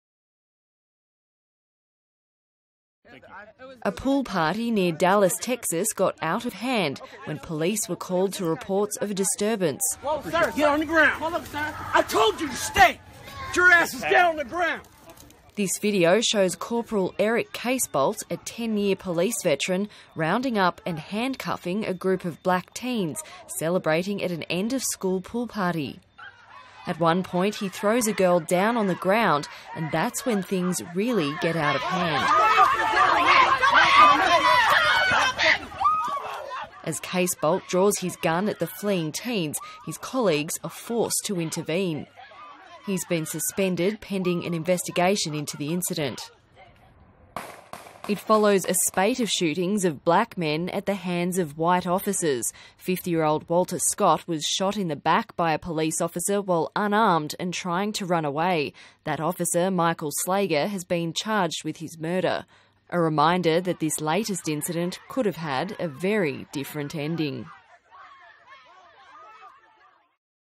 Police officer pulls gun on teens 01:10
A Texas police officer is suspended after drawing his weapon at a group of teens celebrating at an end-of-school pool party.

    Police officer pulls gun on teens
    01:10

    01:10

    01:10